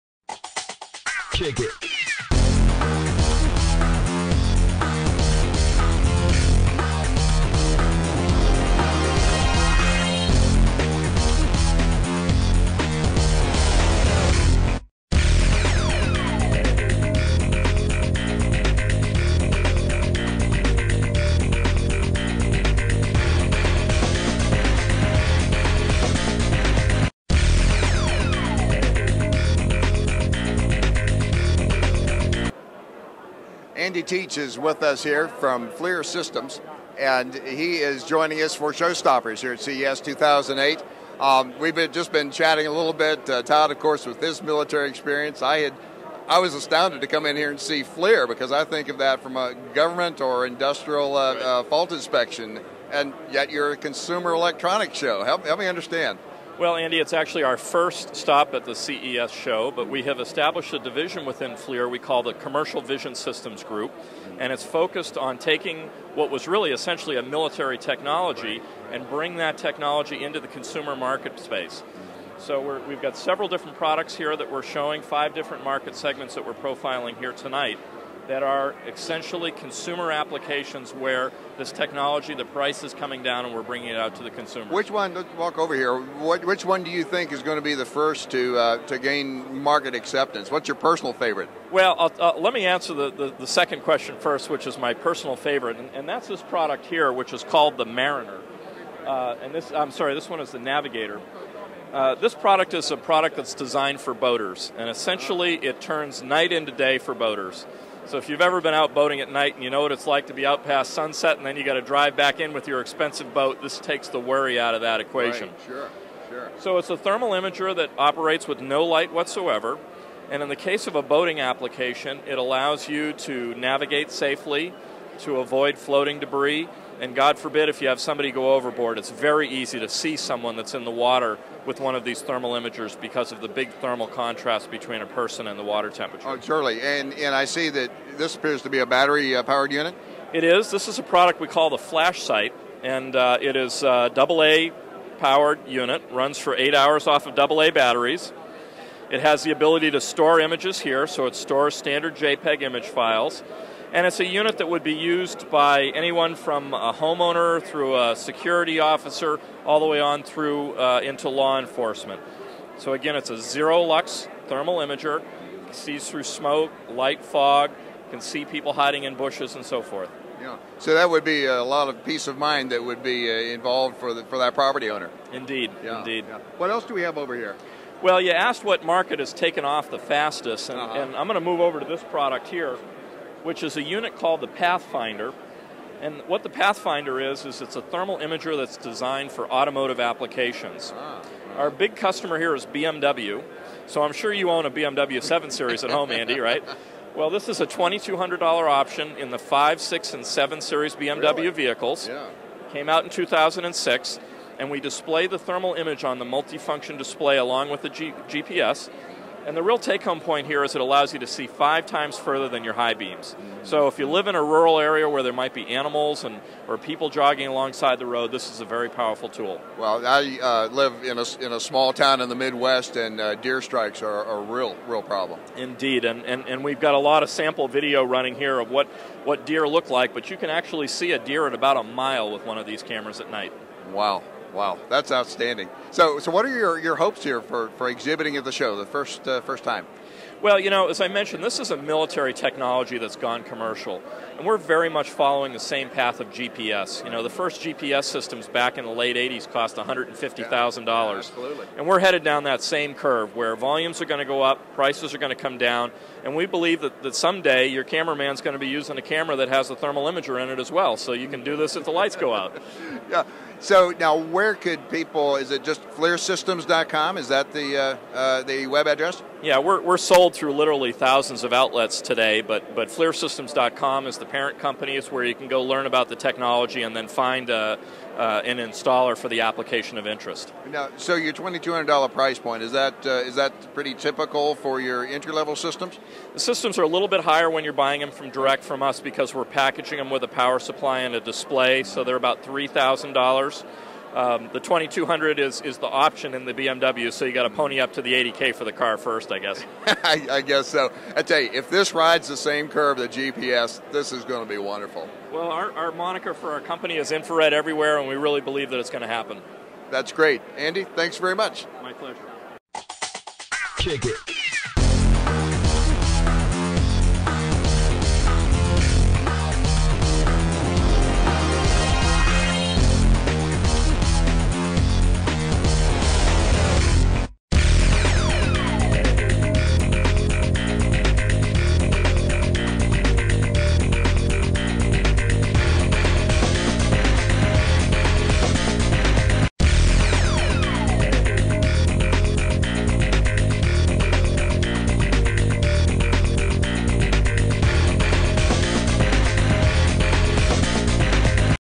CES 2008: Flir Systems now protects your home with ...
Flir Systems is now offering consumer products that allows people to protect their homes with Infrared technology. They have products for Mariners so that people with boats that allow them to navigate safely in the dark. Bringing military technolgy to the home. Not only homes and boats but also those with BMW's can get a Flir imaging system as a option for their car. If you want more information on Flir and info on future special offers please